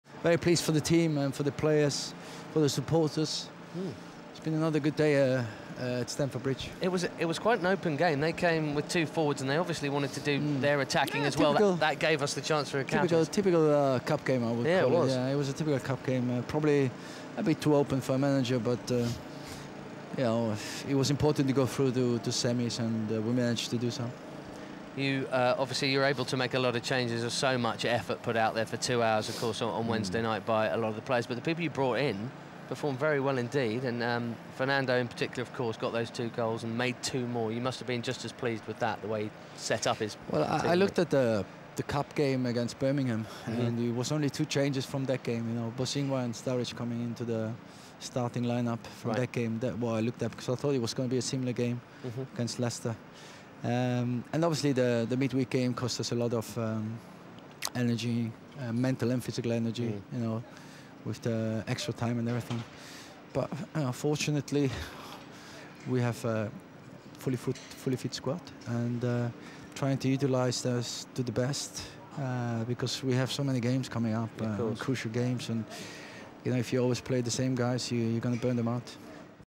Chelsea FC - Di Matteo on Leicester
Roberto Di Matteo spoke to Chelsea TV after the 5-2 win against Leicester in the FA Cup Quarter-Final at Stamford Bridge.